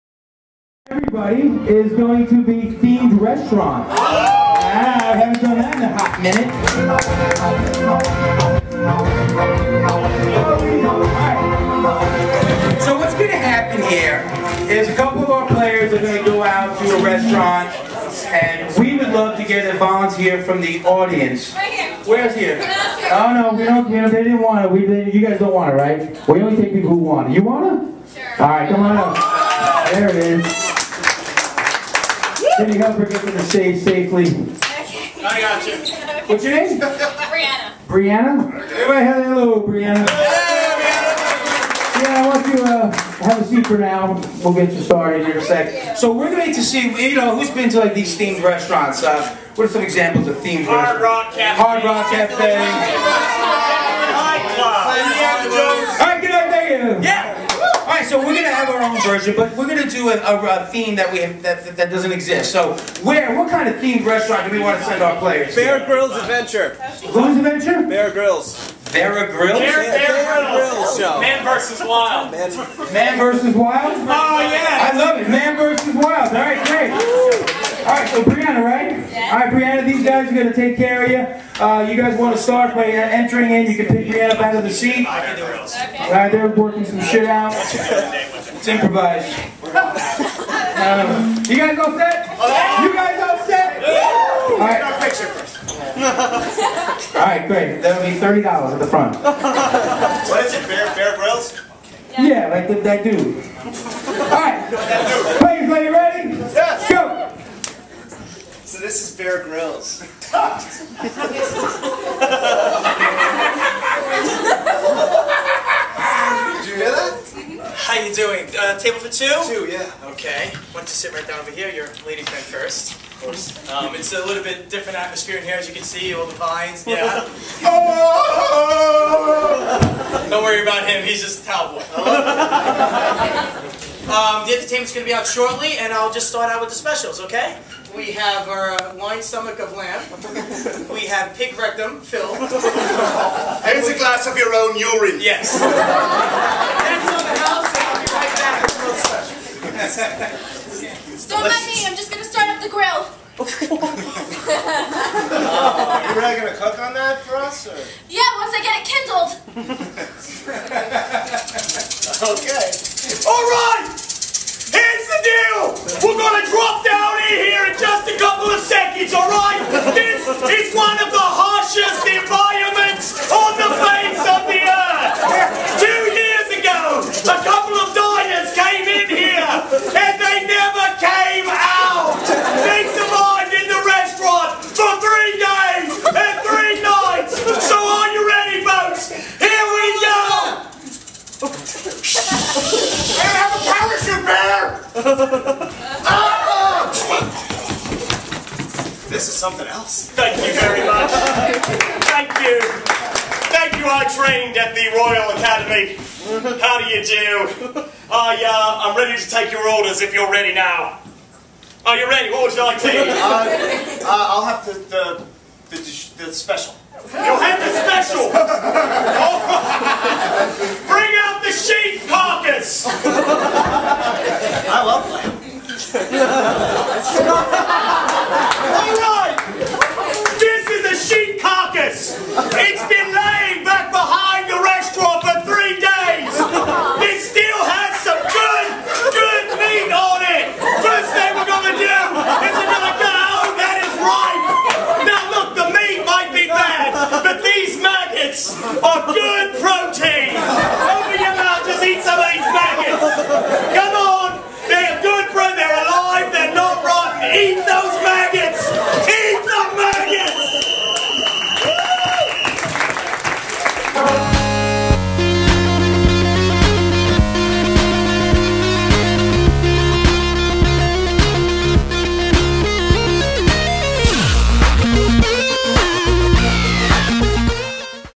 Friday Night Face Off Themed Restaurant (6-18-10)
An actor and an audience member go on a date in a themed restaurant (theme provided by the audience's suggestion